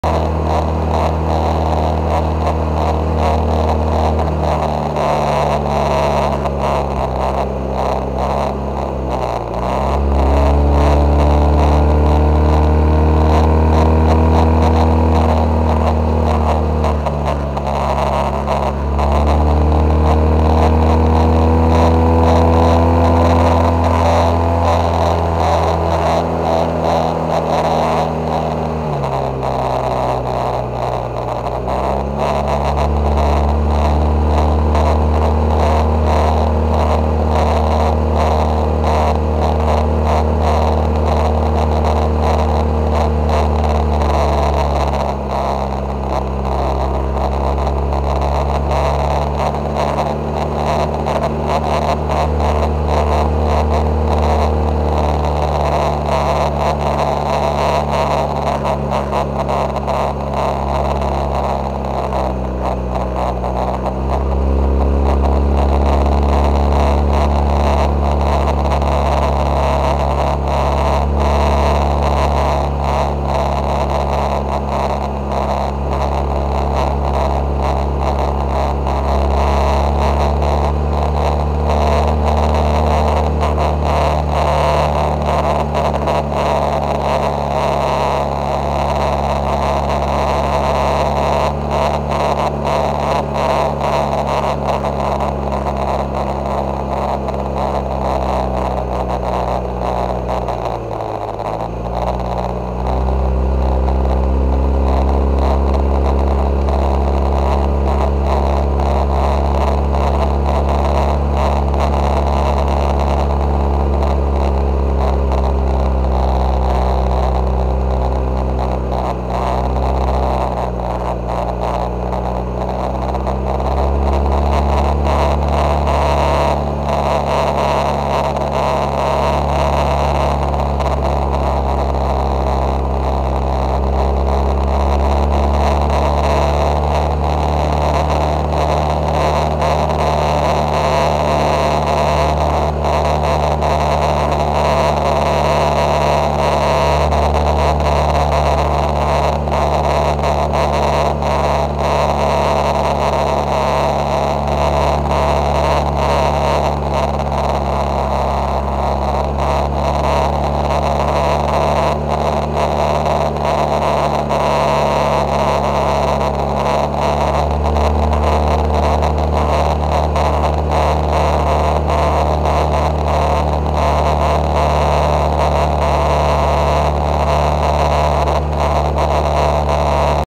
NJ 519` s, # 3, 8/2/23, REC 230802 181618 001049F
County Route 519 s
# 3
Warren County New Jersey
Johnsonburg / Frelinghuysen Tswp to Hope NJ
Aug 2,   2023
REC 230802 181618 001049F
*Bad Microphone,  bad sound*